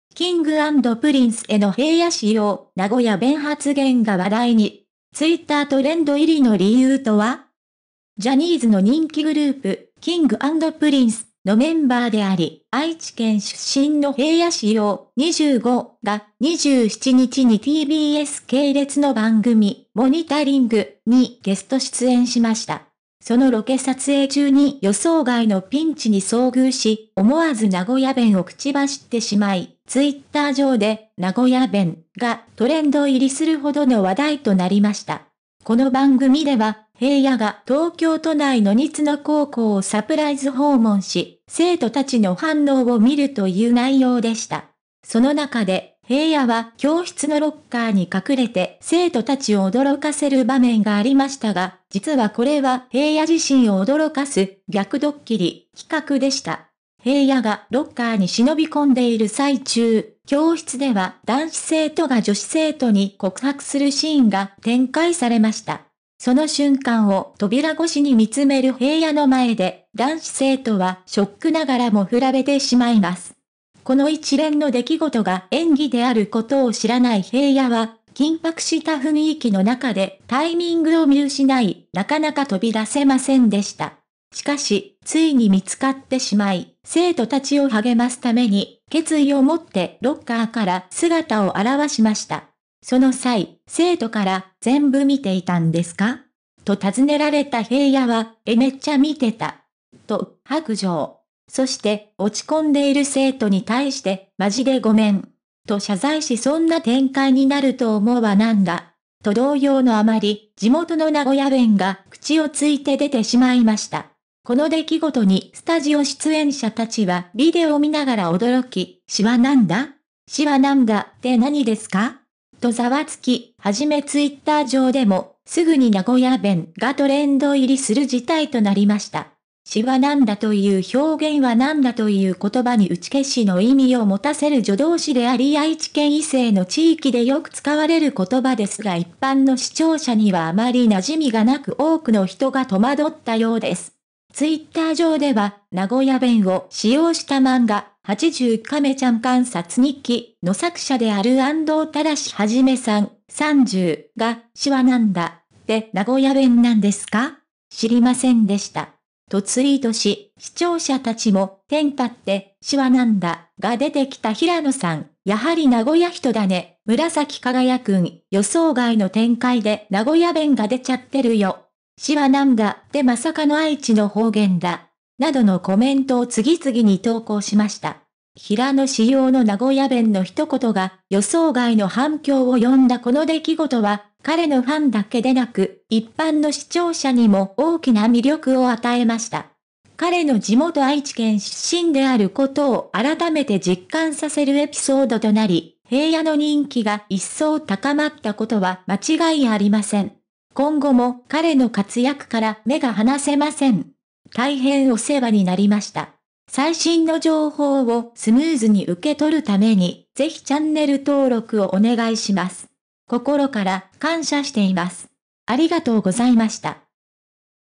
「King & Princeの平野紫耀、名古屋弁発言が話題に！ツイッタートレンド入りの理由とは？」
「King & Princeの平野紫耀、名古屋弁発言が話題に！」